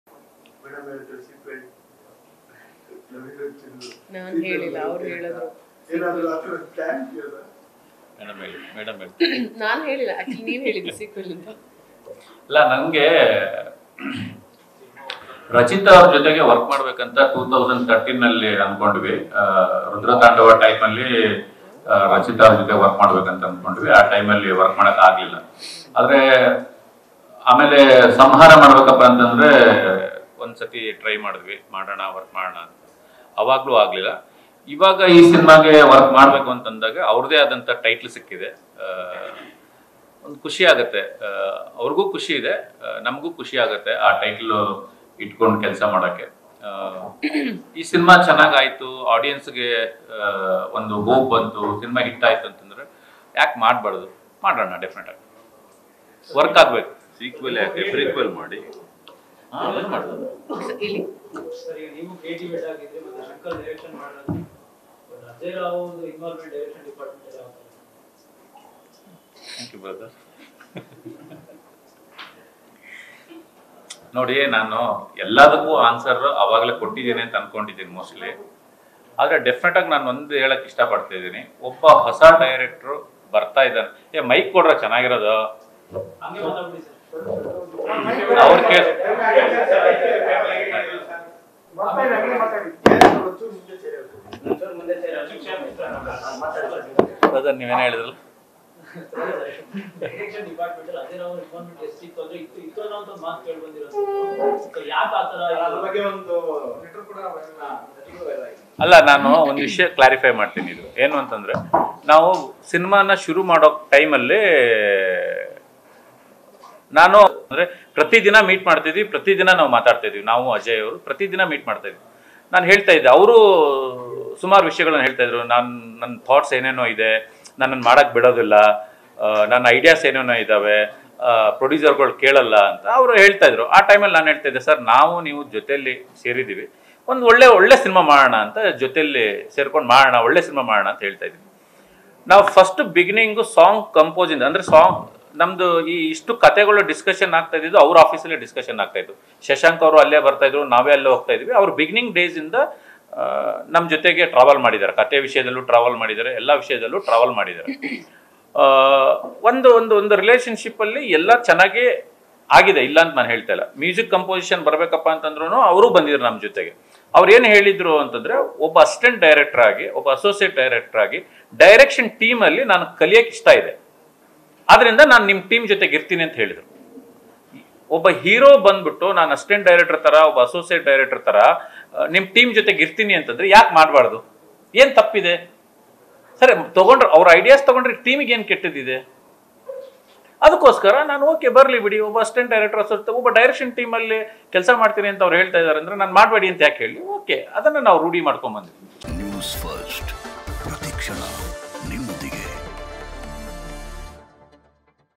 Guru Deshpande : ಅಜಯ್ ರಾವ್ ಡೈರೆಕ್ಷನ್ ಟೀಮ್​ನಲ್ಲಿ ಕೆಲ್ಸ ಮಾಡ್ತೀನಿ ಅಂದ್ರು | Love You Rachchu | NewsFirst
Guru Deshpande : ಅಜಯ್ ರಾವ್ ಡೈರೆಕ್ಷನ್ ಟೀಮ್​ನಲ್ಲಿ ಕೆಲ್ಸ ಮಾಡ್ತೀನಿ ಅಂದ್ರು | Love You Rachchu | NewsFirst Kannada